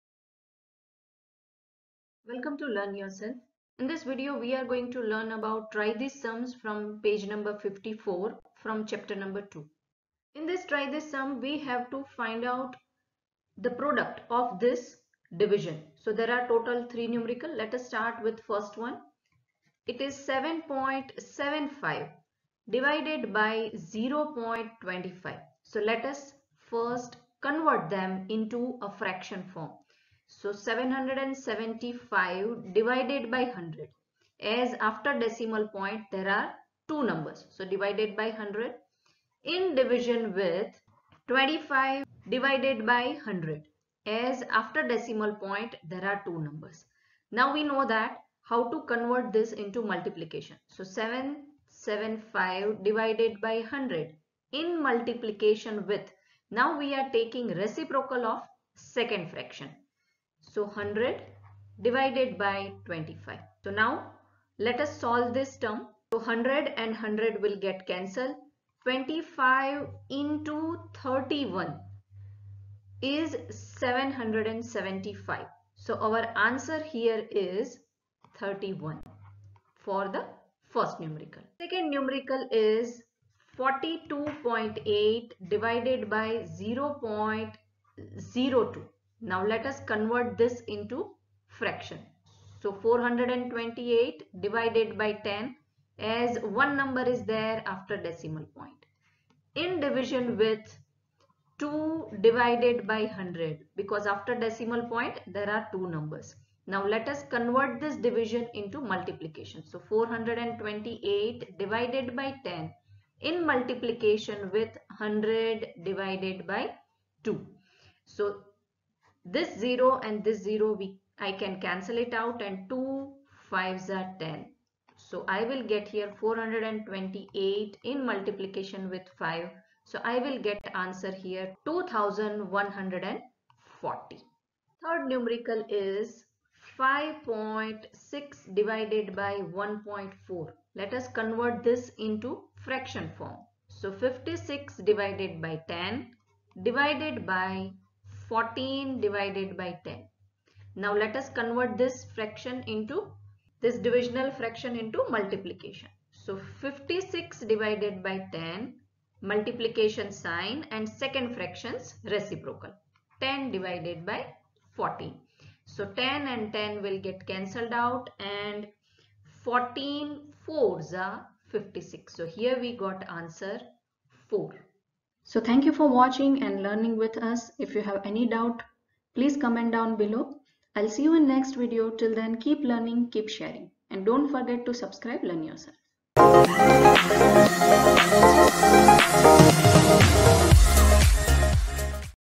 Ch-2 Page no.54 Try These Sums|| CBSE Std-7 Math Ch-2|| Fraction & Decimals
Find: (i)7.75/0.25, (ii)42.8/0.02, (iii)5.6/1.4,